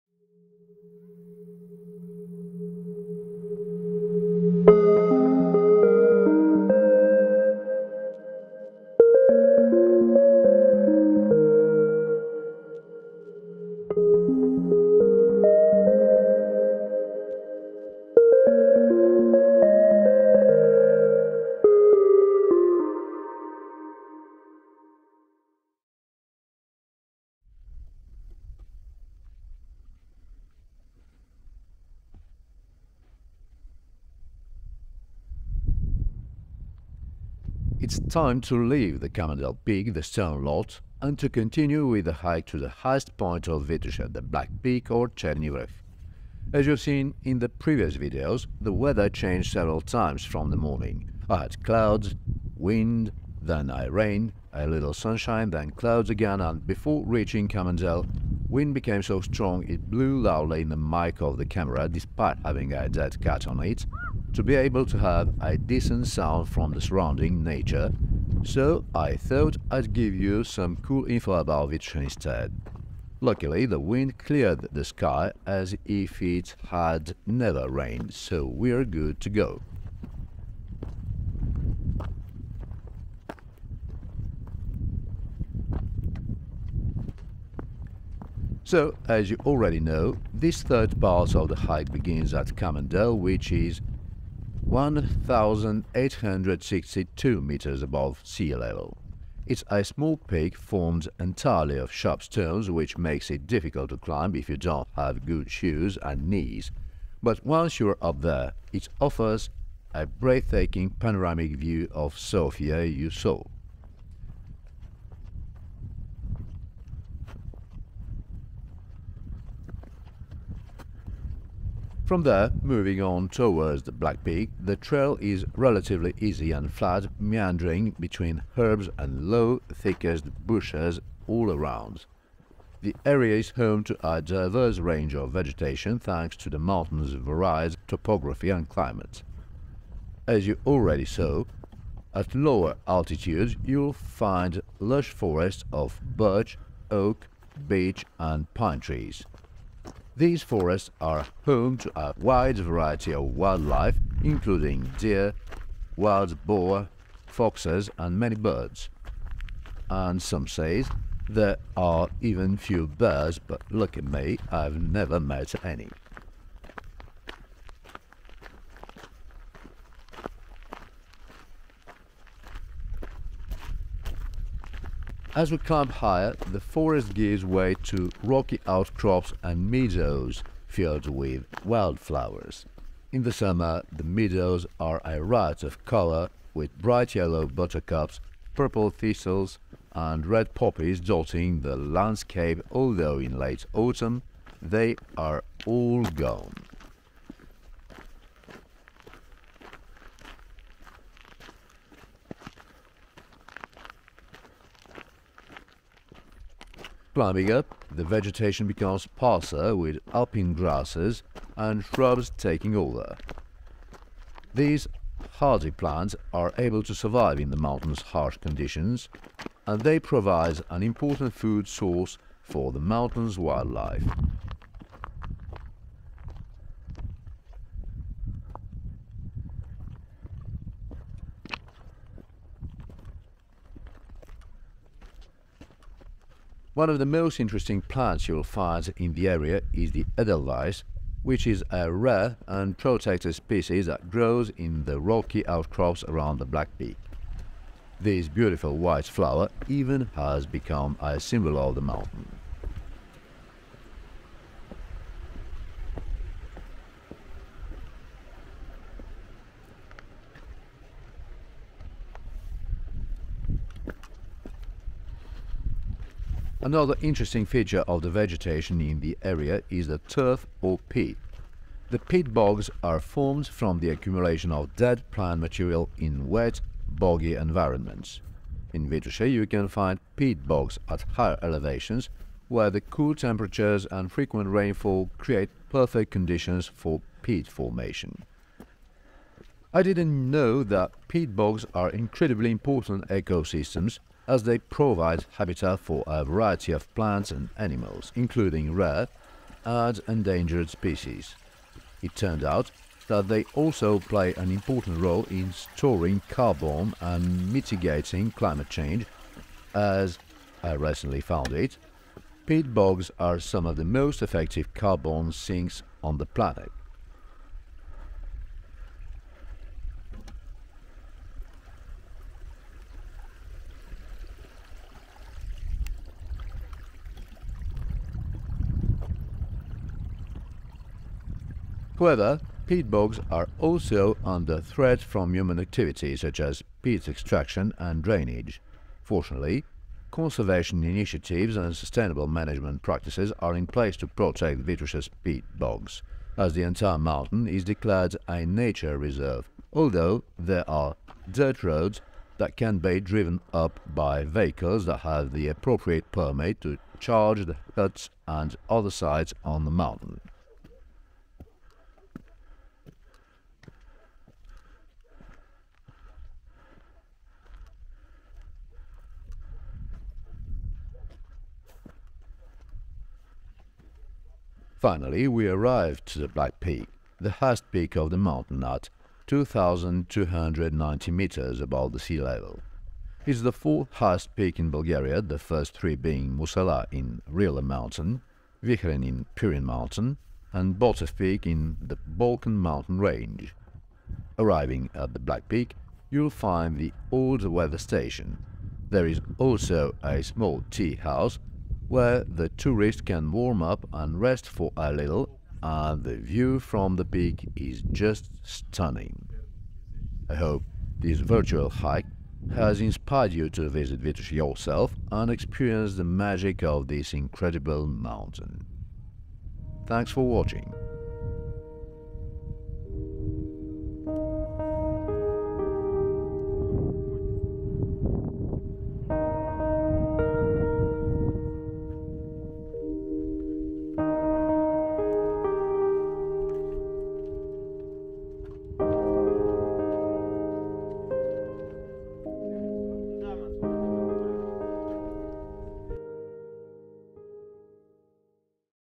A Hike to Black Peak in Vitosha Mountain | Part 3: Kamen Del Peak to Black Peak | 4K/60fps
This is the third part of the Vitosha mountain hike I took from Dragalevski monastery to the Black Peak.

And now it’s time to leave the Kamen Del peak, the Stone Lot, and to continue with the hike to the highest point of Vitosha – the Black Peak or Cherni Vruh. As you’ve seen in the previous videos, the weather changed several times from the morning: I had clouds, wind, then a rain, a little sunshine, then clouds again and before reaching Kamen Del, wind became so strong it blew loudly in the mic of the camera – despite having a dead cat on it – to be able to have a decent sound from the surrounding nature, so I thought I'd give you some cool info about Vitosha instead. Luckily, the wind cleared the sky as if it had never rained, so we're good to go.
So, as you already know, this third part of the hike begins at Kamen Del, which is 1,862 meters (6,109 ft.) above sea level. It’s a small peak formed entirely of sharp stones which makes it difficult to climb if you don’t have good shoes and knees, but once you’re up there, it offers a breathtaking panoramic view of Sofia you already saw.

From there, moving on towards the Black Peak, the trail is relatively easy and flat, meandering between herbs and low, thickset bushes all around. The area is home to a diverse range of vegetation, thanks to the mountain's varied topography and climate. As you already saw, at lower altitudes you'll find lush forests of birch, oak, beech, and pine trees. These forests are home to a wide variety of wildlife, including deer, wild boar, foxes, and many birds. And some says there are even few bears but lucky me, I’ve never met any.

As we climb higher, the forest gives way to rocky outcrops and meadows filled with wildflowers. In the summer, the meadows are a riot of colour, with bright yellow buttercups, purple thistles, and red poppies dotting the landscape although in late autumn they are all gone. Climbing up, the vegetation becomes sparser, with alpine grasses and shrubs taking over. These hardy plants are able to survive in the mountain's harsh conditions, and they provide an important food source for the mountain's wildlife.
One of the most interesting plants you'll find in the area is the edelweiss, which is a rare and protected species that grows in the rocky outcrops around the Black Peak. This beautiful white flower even has become a symbol of the mountain.

Another interesting feature of the vegetation in the area is the turf or peat. The peat bogs are formed from the accumulation of dead plant material in wet, boggy environments. In Vitosha, you can find peat bogs at higher elevations, where the cool temperatures and frequent rainfall create perfect conditions for peat formation.

I didn’t know peat bogs are incredibly important ecosystems, as they provide habitat for a variety of plants and animals, including rare and endangered species. It turned out that they also play an important role in storing carbon and mitigating climate change, as – I recently found it – peat bogs are some of the most effective carbon sinks on the planet.
However, peat bogs are also under threat from human activities, such as peat extraction and drainage. Fortunately, conservation initiatives and sustainable management practices are in place to protect Vitosha's peat bogs, as the entire mountain is declared a nature reserve, although there are dirt roads that can be driven up by vehicles that have the appropriate permit to charge the huts and other sites on the mountain.

Finally, we arrive to the Black Peak – the highest peak of this mountain – at 2,290 meters (7,513 ft.) above the sea level. It’s the fourth highest peak in Bulgaria, the first three being Mussala in Rila mountain (2,925 m. high / 9,596 ft.), Vihren in Pirin mountain (2,914 m. / 9,560 ft.) and Botev peak in the Balkan Mountain range (2,376 m. / 7,795 ft.). Arriving at the Black Peak you’ll find the old weather station. There is also a small teahouse where the tourists can warm up and rest for a little, and the view from the peak is just stunning.

I hope this virtual hike has inspired you to visit Vitosha yourself and experience the magic of this incredible mountain.